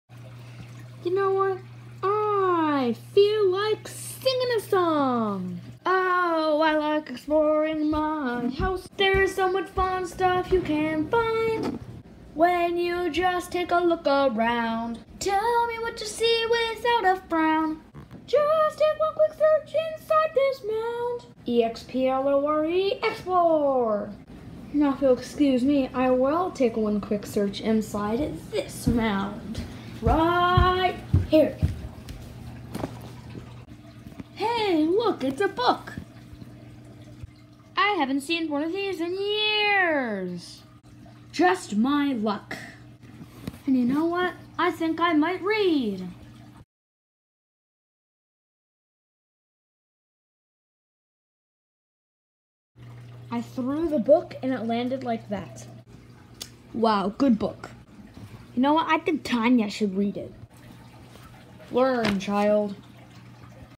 Random Stuffed Animal Shorts Episode 14: Look Its A Book
Harvey finds a book!